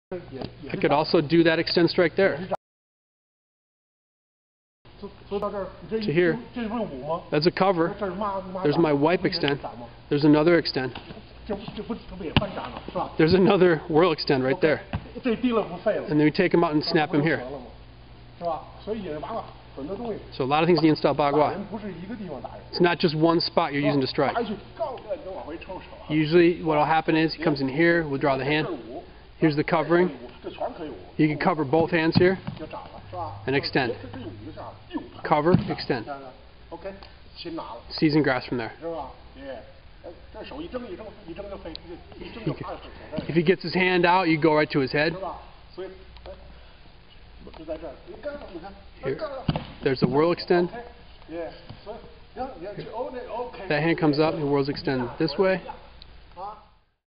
Fighting Arts - Yin Style Bagua - Extending Attacks
Yin Style Baguazhang is an art of striking while moving. It employs intricate kicking, throwing, and grasping techniques which are set up by strikes executed as the practitioner moves around the opponent in a circular fashion.